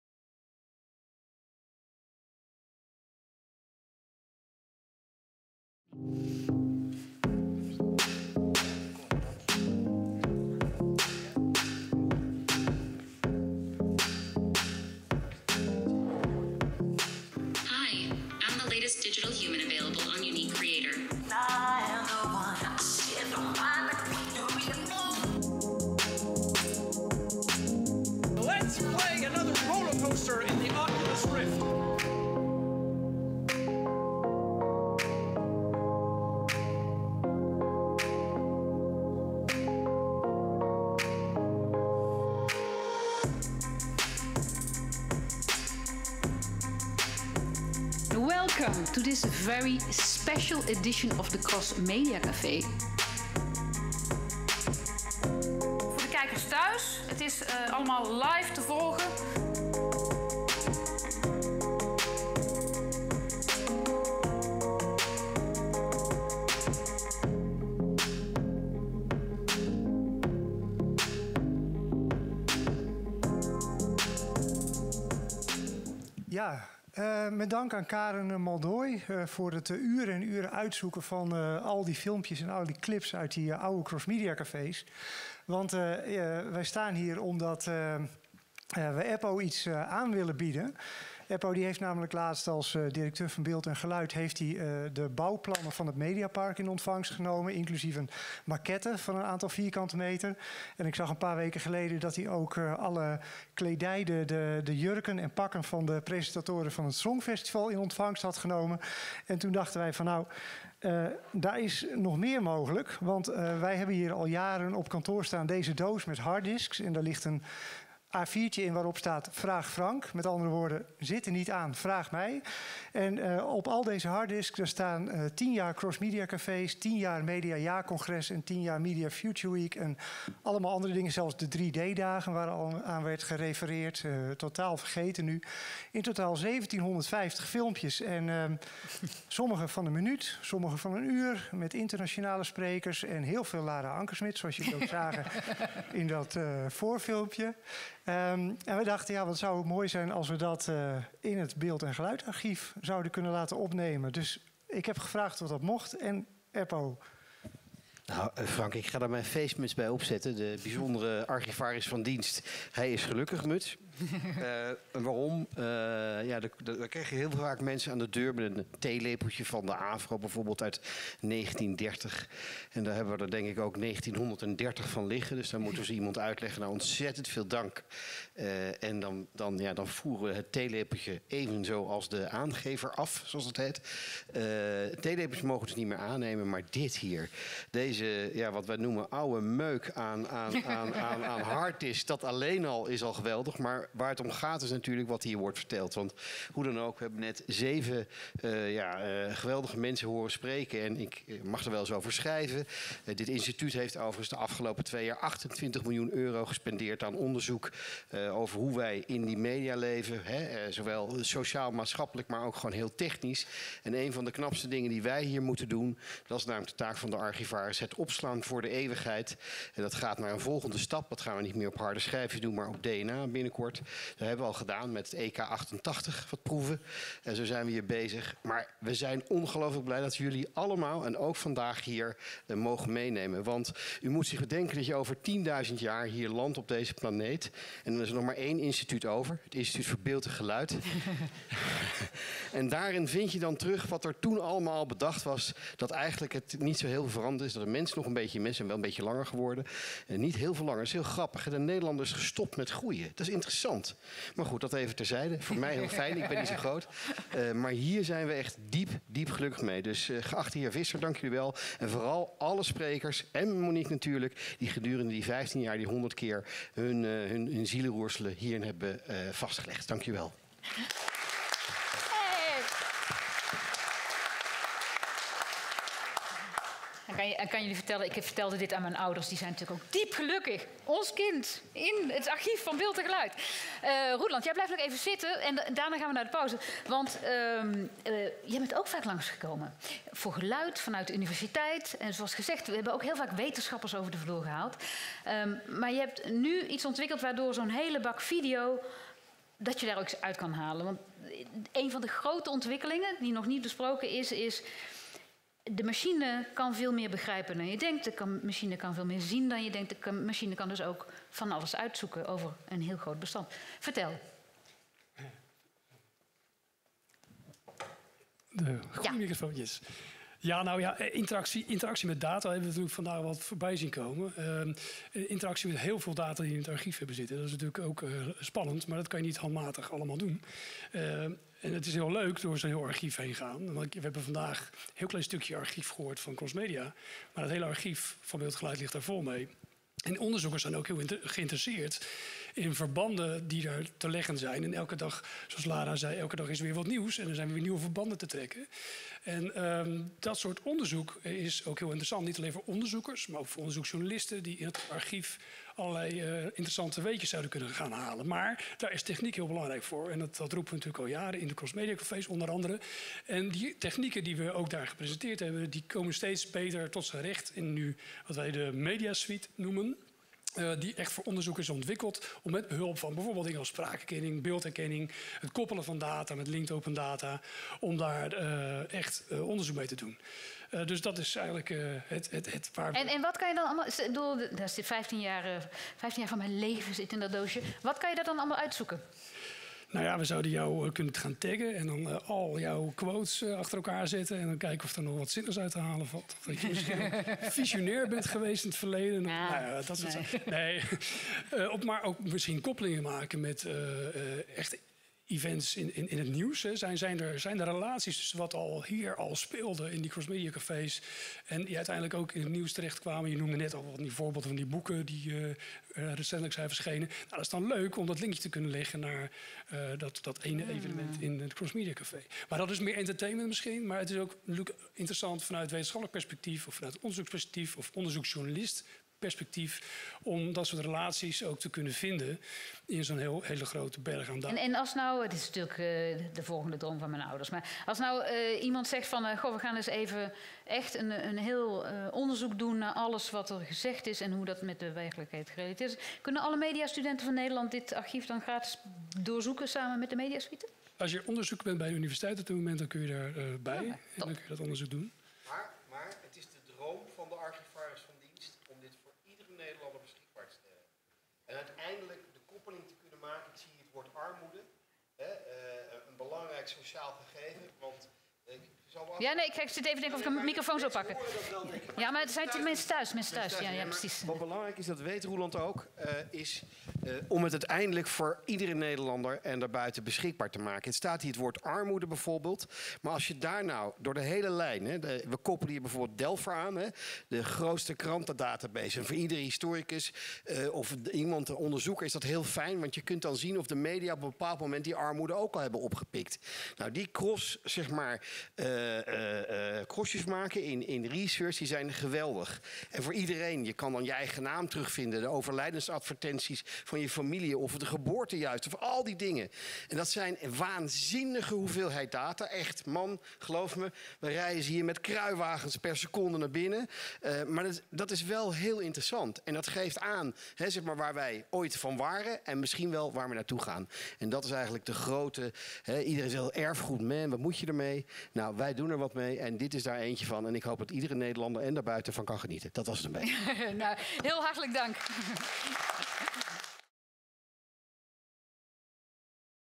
Cross Media Café: Toekomstdromen | Opname videocollectie in de Beeld en Geluid Media Suite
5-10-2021 - Tijdens deze speciale editie van ons 100ste Cross Media Café, die we organiseren in samenwerking met Beeld en Geluid, gaan we terug in de tijd en kijken we vooruit naar de toekomst! Monique van Dusseldorp, sinds 2005 onze vaste samensteller en moderator, gaat in gesprek met adverteerders, uitgevers, mediaondernemers en wetenschappers. Zij schoven de afgelopen vijftien jaar aan om te vertellen over hun innovaties en toekomstdromen. Hoe kijken zij daar op terug? En wat is daarvan terecht gekomen? Natuurlijk zit er ook een nieuwe generatie aan tafel waarmee we vooruit gaan kijken. Hoe ziet het medialandschap er over vijftien jaar uit? Wat moeten we doen om een gezonde, relevante mediasector te behouden? Wat zijn de dromen voor de toekomst?

In deze video komen de volgende gasten aan het woord:
- Frank Visser (Media Perspectives)
- Eppo van Nispen tot Sevenaer (Beeld en Geluid)
- Roeland Ordelman (Beeld en Geluid)